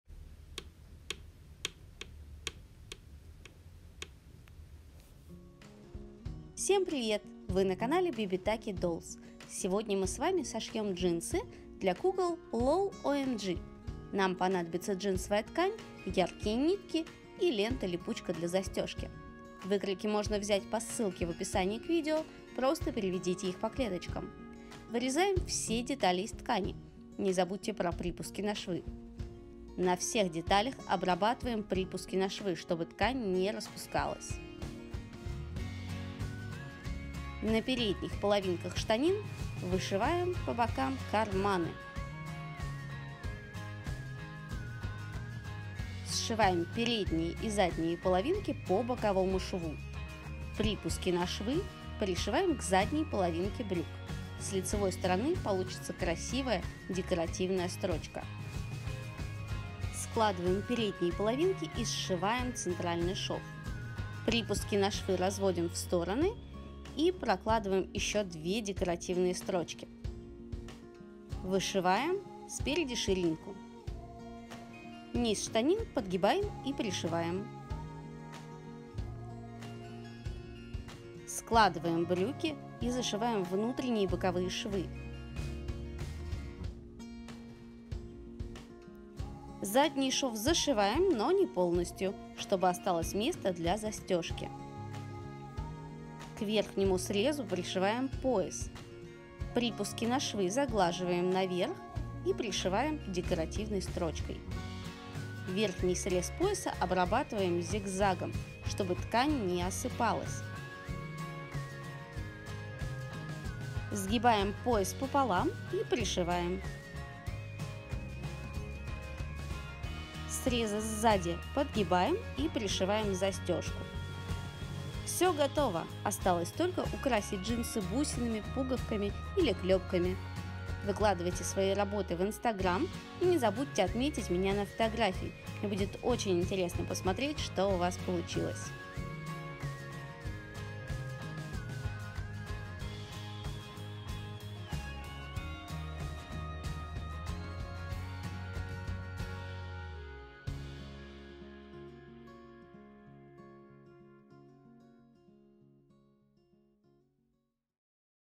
DIY Как сшить джинсы для кукол LOL OMG. Мастер-класс по шитью. Бесплатная выкройка!
Мастер-класс как сшить джинсы для кукол LOL OMG ЛОЛ ОМГ